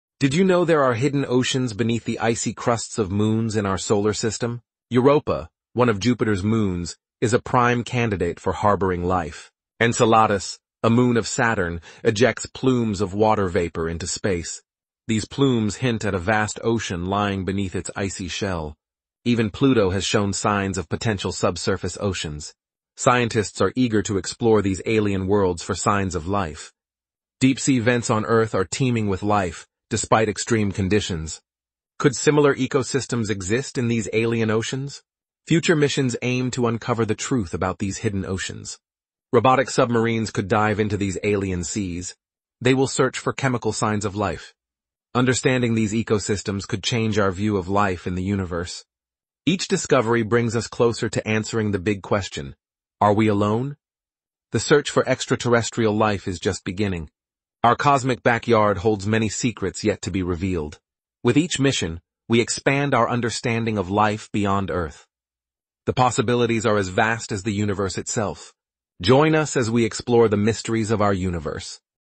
🌊 The Secret Oceans of Our Solar System – Could They Harbor Alien Life? 🌌
Beneath the icy crusts of moons like Europa, Enceladus, and even Pluto, vast hidden oceans may be teeming with secrets—and possibly life! Scientists believe these extraterrestrial waters could resemble Earth's deep-sea environments, where life thrives in complete darkness. Could these hidden oceans hold the key to finding alien organisms? How do we plan to explore these mysterious worlds? Join us as we dive deep into the search for life beyond Earth! 🚀🔬🌍